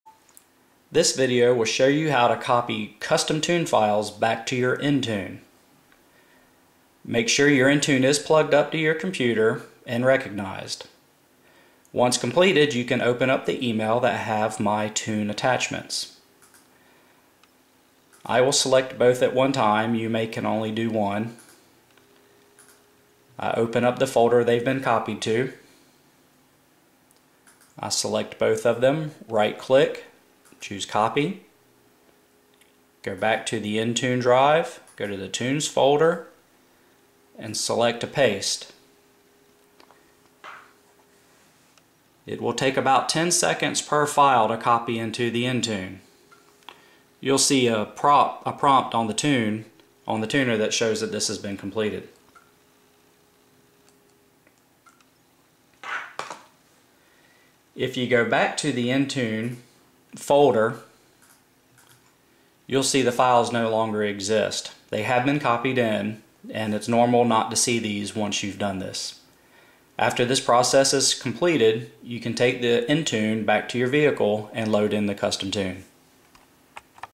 Copy Custom Tunes to Diablo Intune
Shows you how to copy a custom tune from email to your Diablo Intune